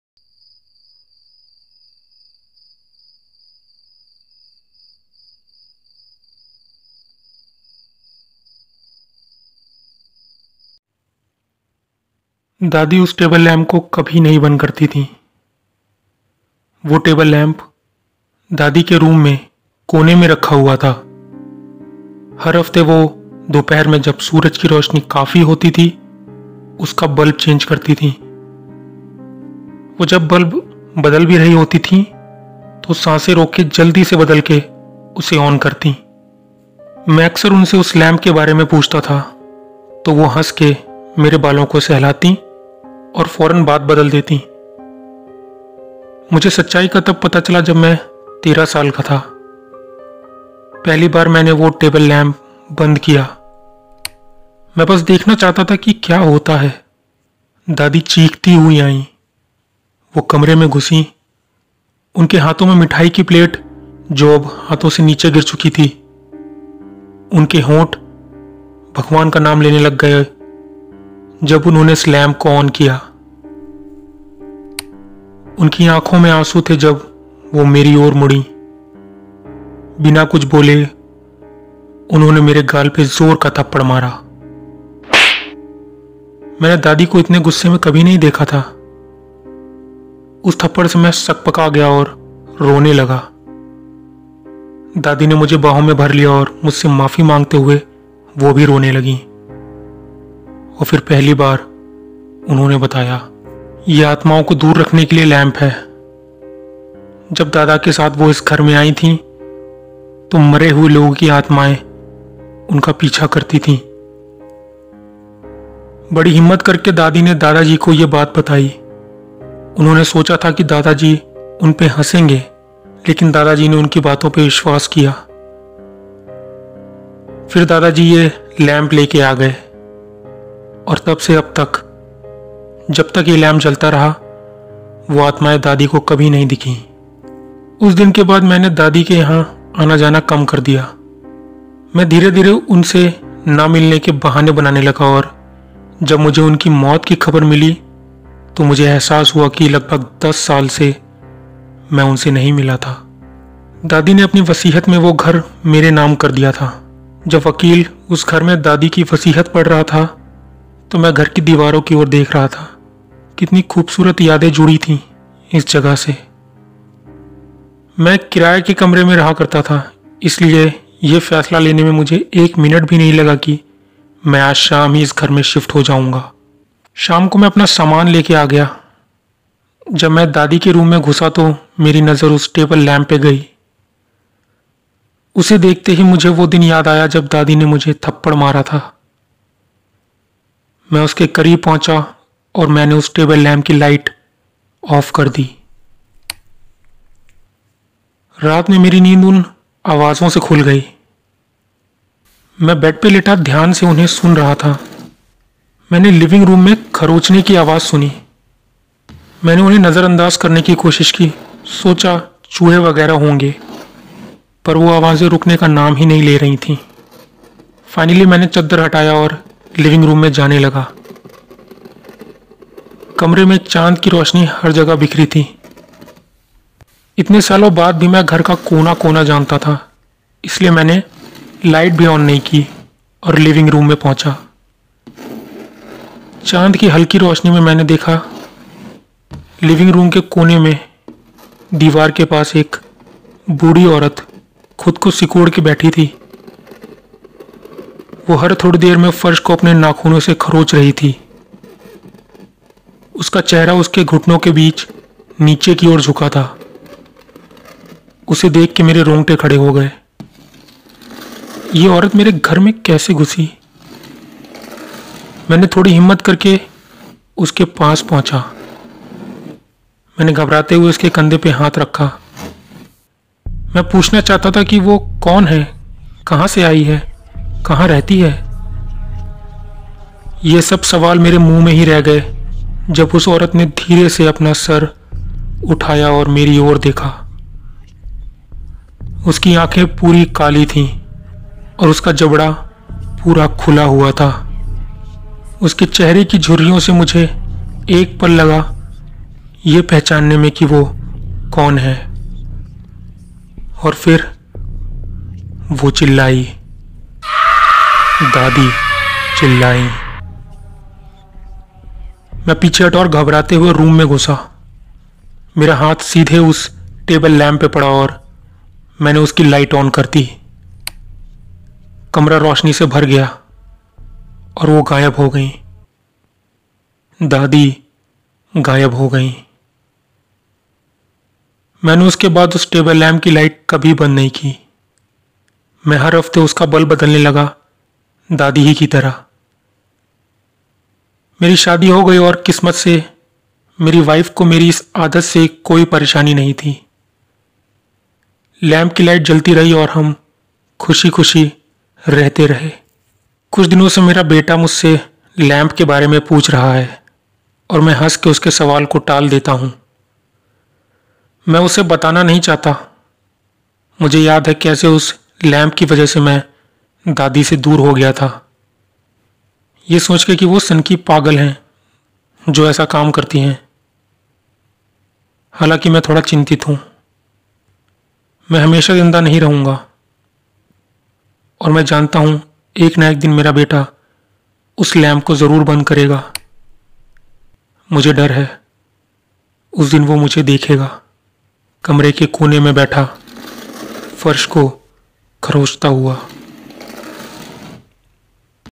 Table Lamp || Horror Suspense Story || Hindi
Never switch off the table lamp. Put your headphones on and enjoy.

डरावनी कहानियां
हिंदी हॉरर स्टोरी
Bhooto ki Kahani